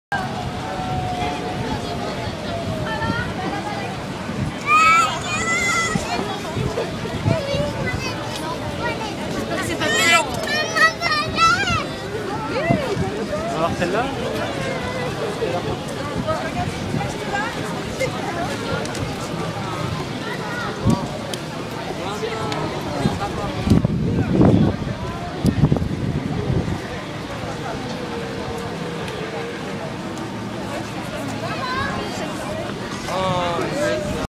The Paris Air Show -2013, Le Bourget
The Paris Air Show (Salon international de l'aéronautique et de l'espace, Paris-Le Bourget) claims to be the world's oldest and largest air show. Established in 1909, it is currently held every odd year at Le Bourget Airport in north Paris, France. The 2013 Paris Air Show, on the 17th to the 23rd of June 2013, became the 50th.